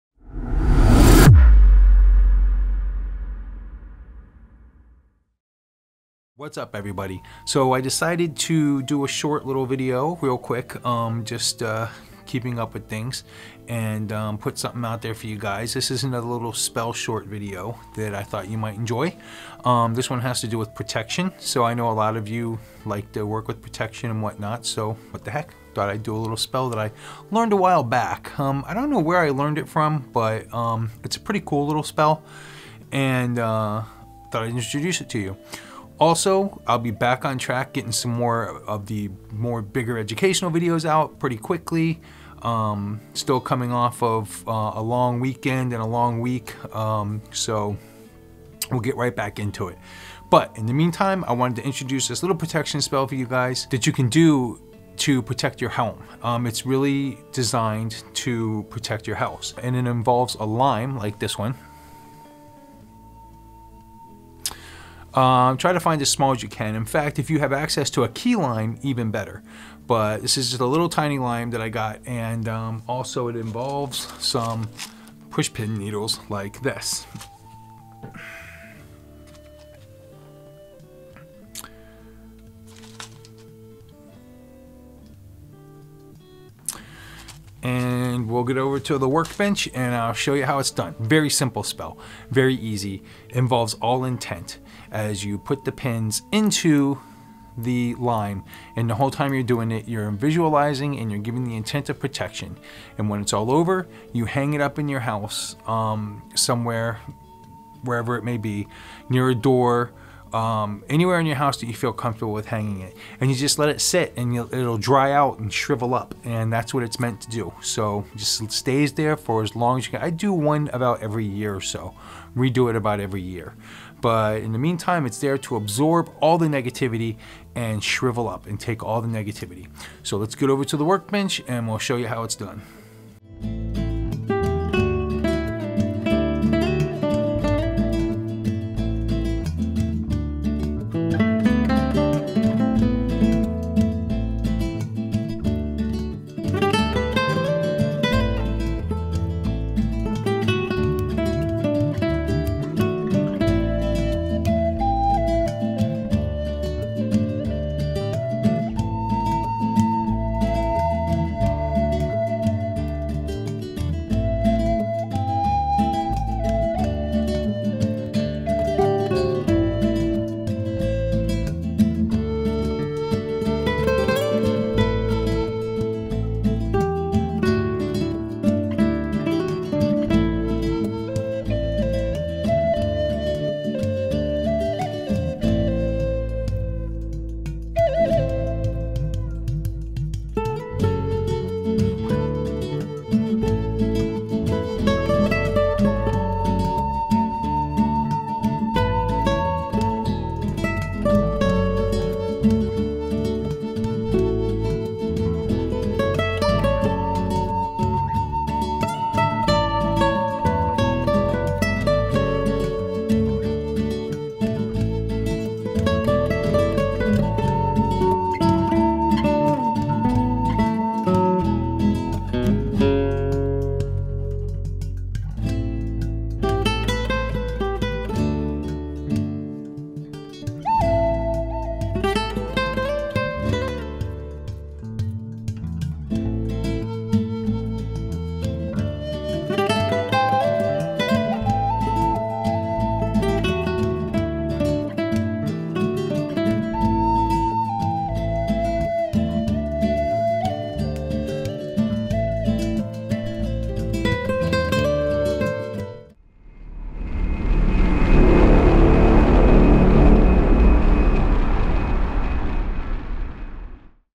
Lime Protection Spell
A Protection Spell for the Home!!

Ship any Packages to:
Mannun
497 Hooksett Rd. #185

Also to see Traditional Gardnerian Covens in your area

Weeping Willow Coven